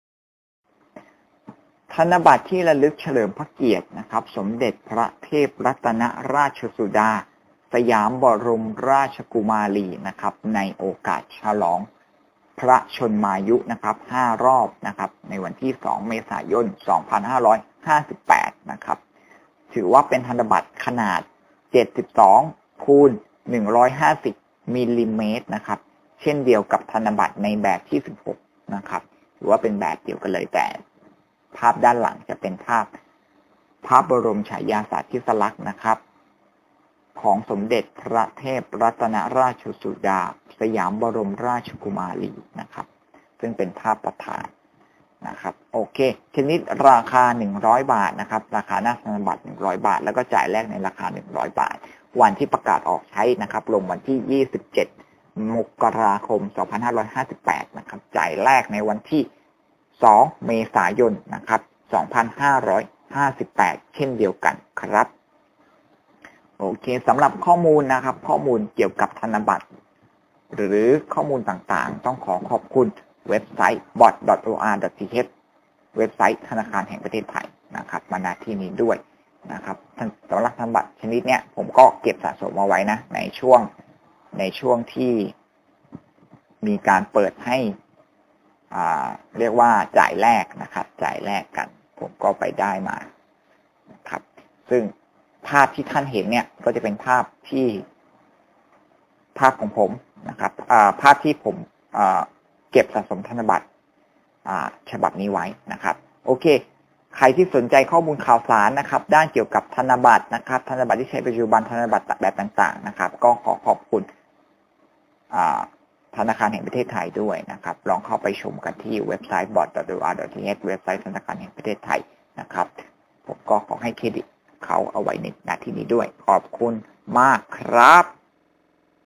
ธนบัตรที่ระลึก ep11 เฉลิมพระเกียรติสมเด็จพระเทพรัตนราชสุดาฯ | สะสมธนบัตรเก่า แบงค์เก่าหายาก
​​​​ธนบัตรที่ระลึกเฉลิมพระเกียรติสมเด็จพระเทพรัตนราชสุดาฯ
สยามบรมราชกุมารี ในโอกาสฉลองพระชนมายุ 5 รอบ 2 เมษายน 2558